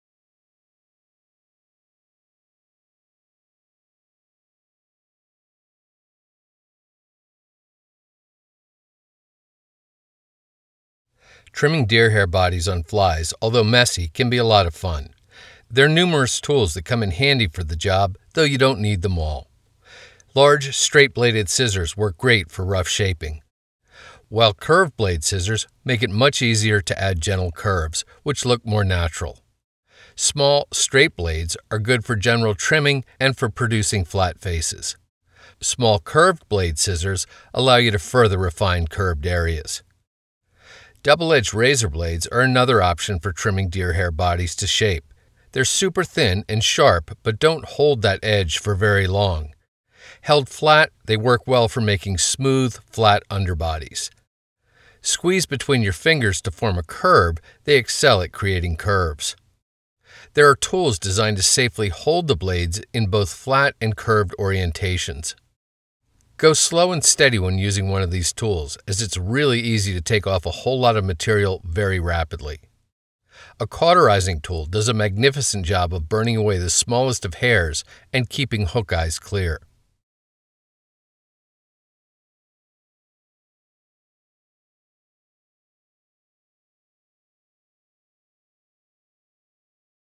Trimming Deer Hair
Video shows different tools and methods for trimming and shaping deer hair bodies on flies.